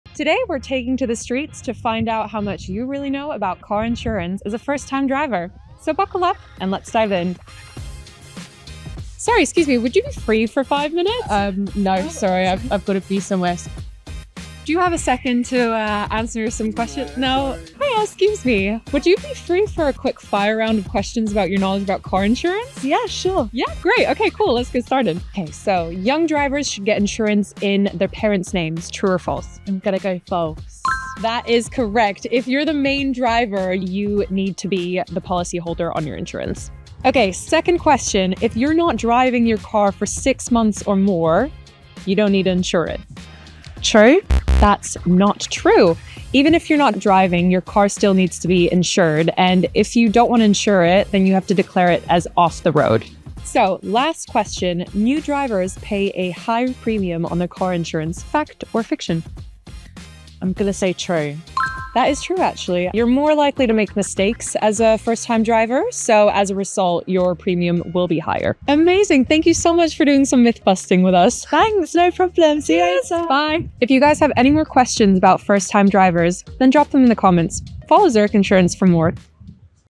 Discover the myths around car insurance
Once you’ve passed your driving test or even if you’re still learning, you’ll need to think about getting car insurance! But are you sure know everything you need to get properly insured? 🚗